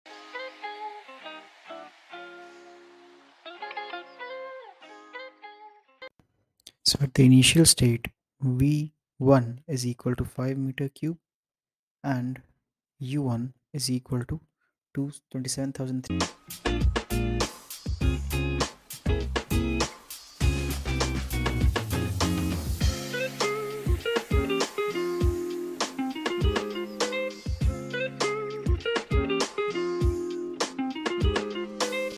A 5 m3 vessel initially contains 50 kg of liquid water at 480.24 K and saturated water vapor at tot…
A 5 m3 vessel initially contains 50 kg of liquid water at 480.24 K and saturated water vapor at total internal energy of 27,300 kJ. Calculate the heat requirement to vaporize all of the liquid.a) 100,000 kJ b) 200,000 kJ c) 300,000 kJ d) 400,000 kJ